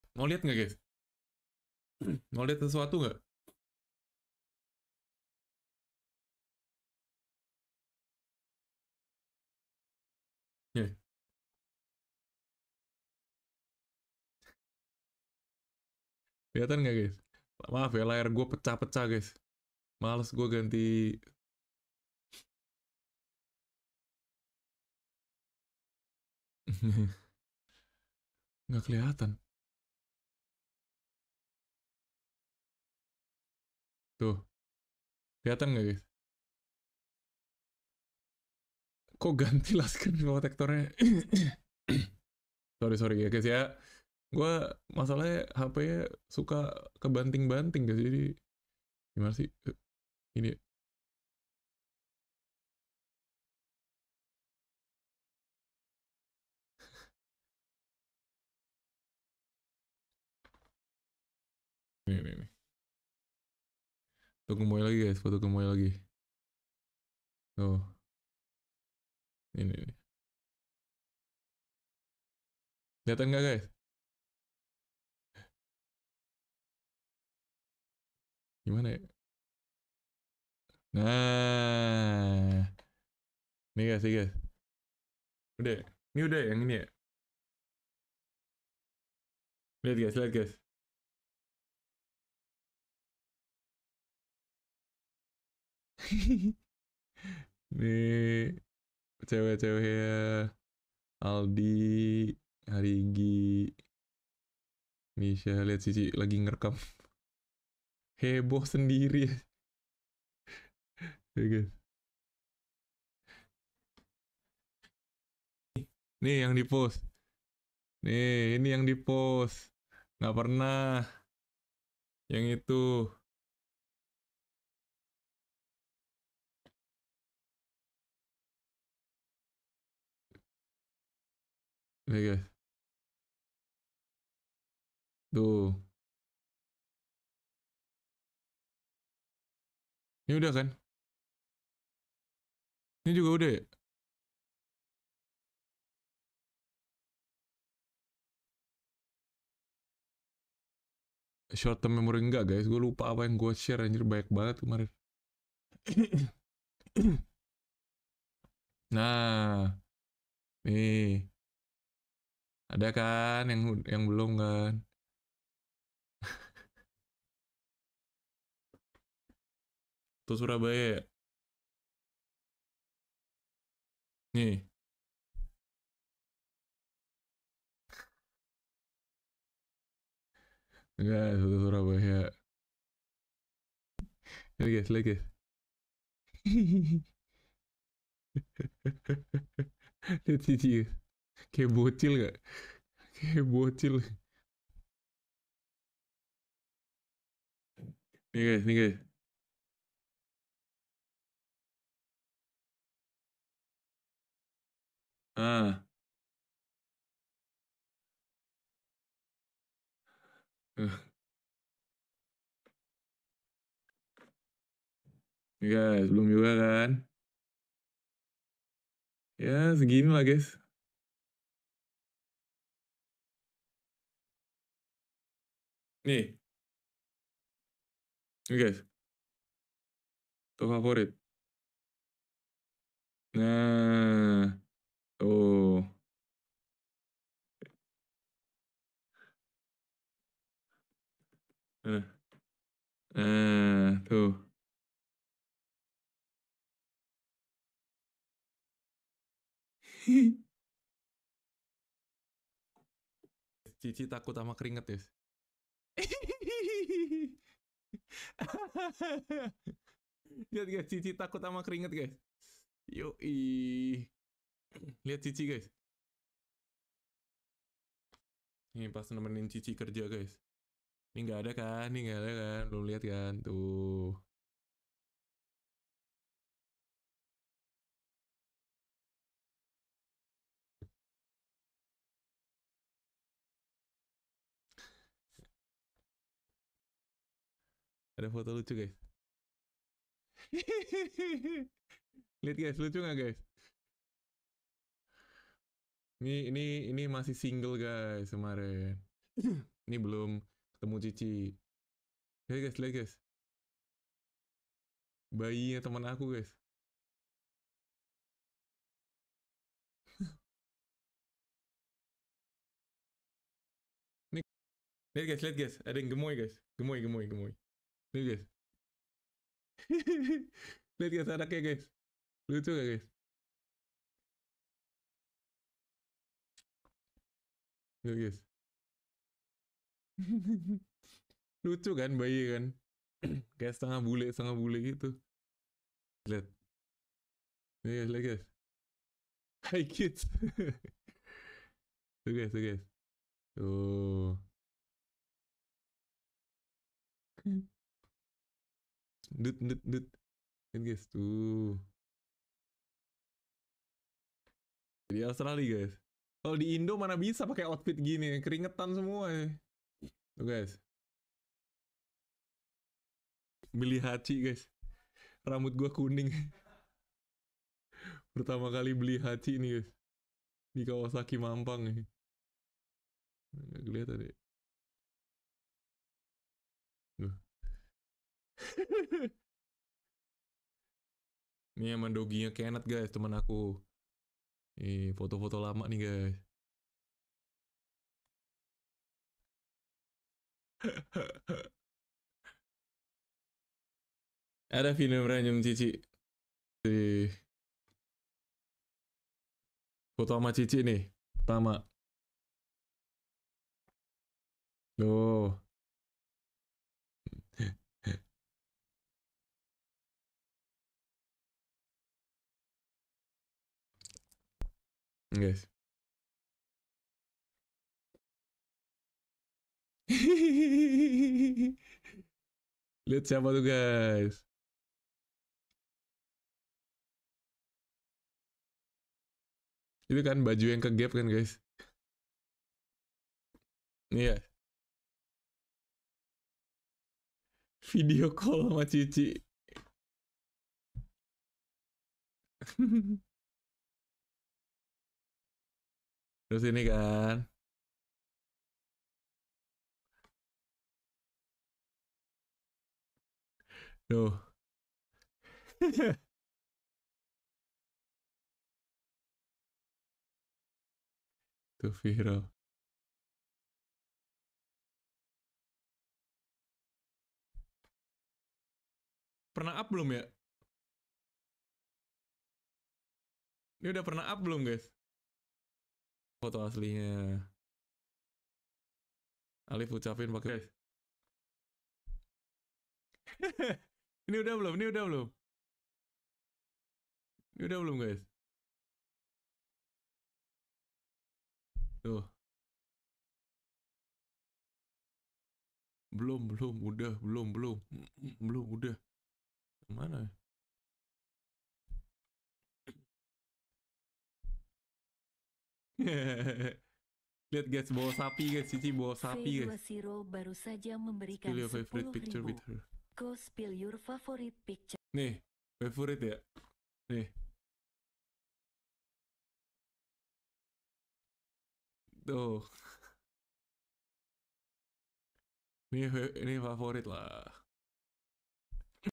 SPAL SPIL FOTO KOKO BARENG CICI || Full nya di live Youtube koko guys 😍
mana semalem cici nelpon koko cuma gara2 kepo di bikin penasaran sama komenan😂🤏